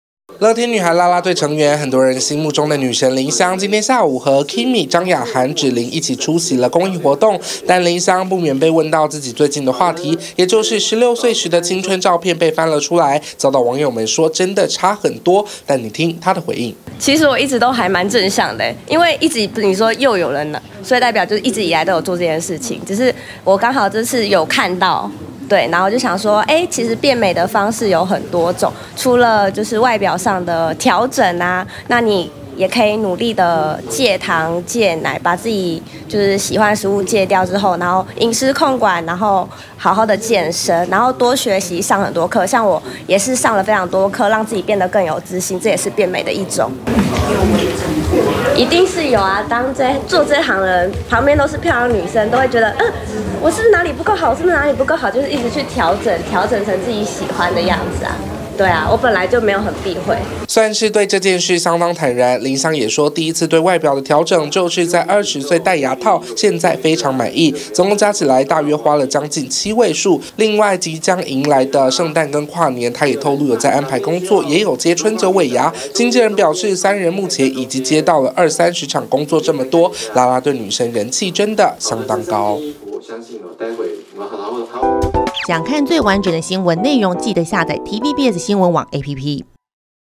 【星聞爆爆】10年前舊照差超多? 林襄露面回應認「微整」
2013年TVBS歡樂台第二部原創概念劇